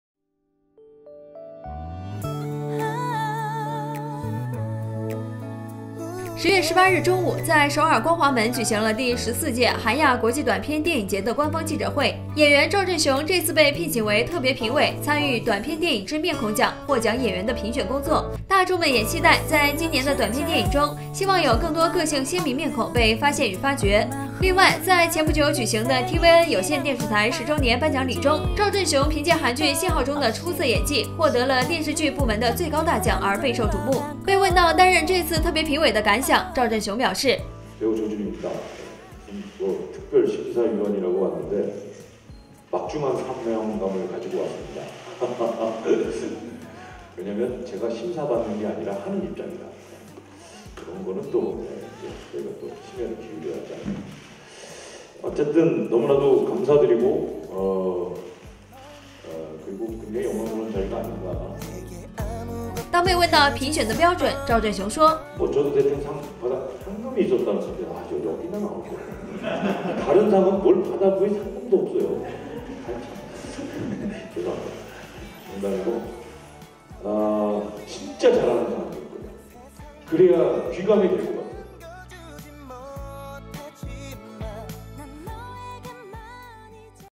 【3年前】趙震雄任短篇電影節特別評委 西裝革履出席展成熟魅力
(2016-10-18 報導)

10月18日中午在首爾光華門，舉行了，第14屆韓亞國際短篇電影節的官方記者會。演員趙震雄這次被聘請爲特別評委，參與“短片電影之面孔獎” 獲獎演員的評選工作。
大衆們也期待在今年的短片電影中，希望有更多個性鮮明面孔被發現與發掘。另外，在前不久舉行的tvN有線電視台10週年頒獎禮中，趙震雄以在韓劇《訊號》中的出色演技獲得了，電視劇部門最高大獎而備受矚目。趙震雄通過電影《狩獵》與“亞洲國際短片電影節”執行委員長安聖基有過合作。同時也出席參加過上屆電影節開幕式活動。